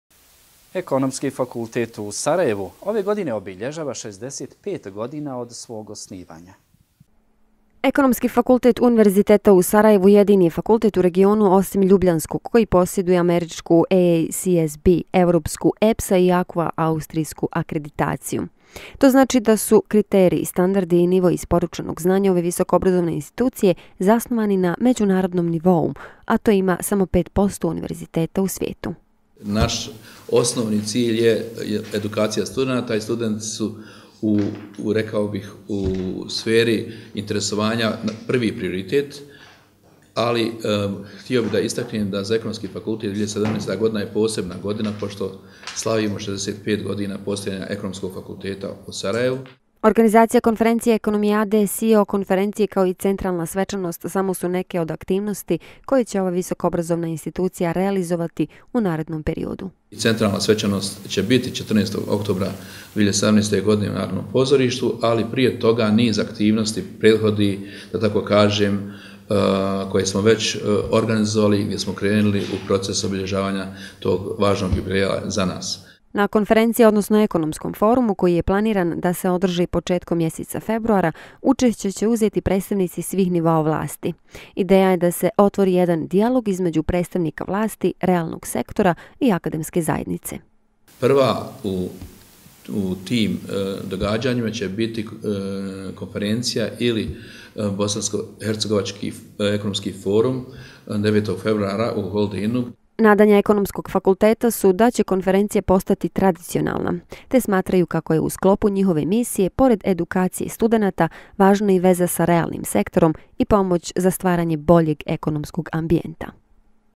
EFSA obilježava 65. godina od svog osnivanja, Kemal Kozarić - STUDENTSKI IMPULS 35
Ekonomski fakultet ove godine obilježava jubilej 65. godina od svog osnivanja, govori prof. dr. Kemal Kozarić - AMT STUDIO